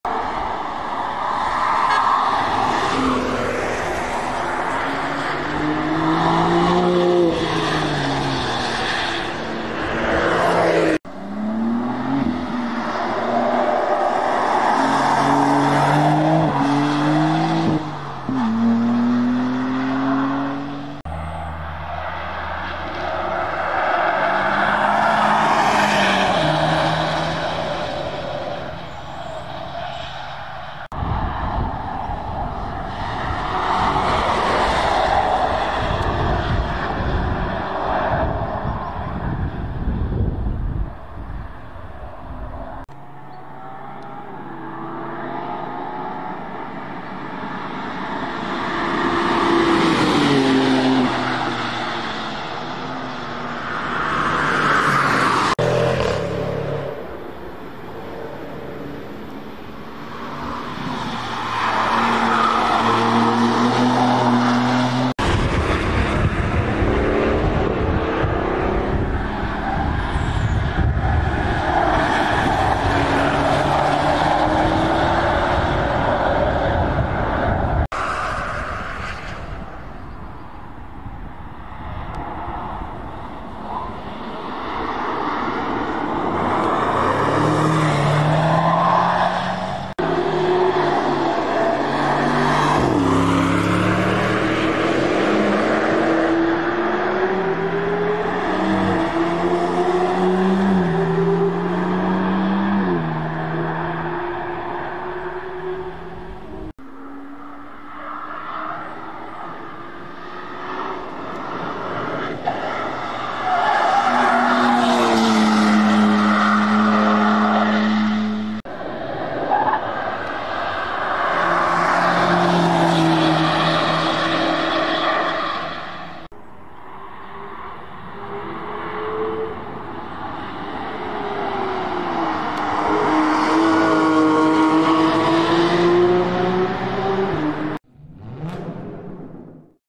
2025 BMW M2 CS, 3.0 CSL Hommage, X5 M Facelift, and M5 at Nürburgring
Various BMW car models have been spotted testing at the Nurburgring, showcasing the manufacturer's continuous efforts in improving and refining their vehicles.

2025 BMW M2 CS will be revealed soon.

BMW 3.0 CSL Hommage already revealed, but we wanted to showcase its exhaust sound.

2024 BMW X5 M Facelift is already revealed and we wanted to showcase its exhaust sound.

And the much anticipated 2025 BMW M5.